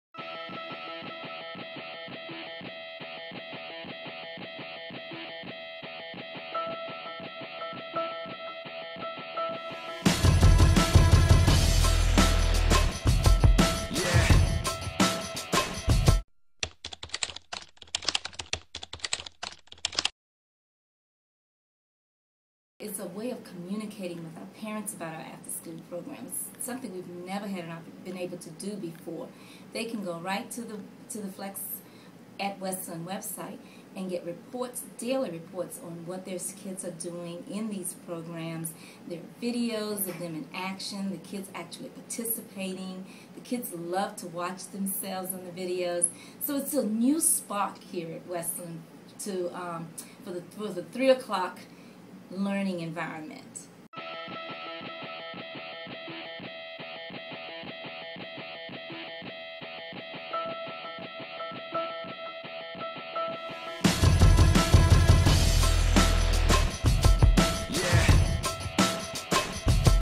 Administration Feedback: Activity reports provide excellent communication to parents
Did you know that everyday your child attends an activity at the Flex Academies you will receive a report detailing what went on that day in the activity.  Assistant Administrator Shelia Harrison talks about the reports and their positive impact.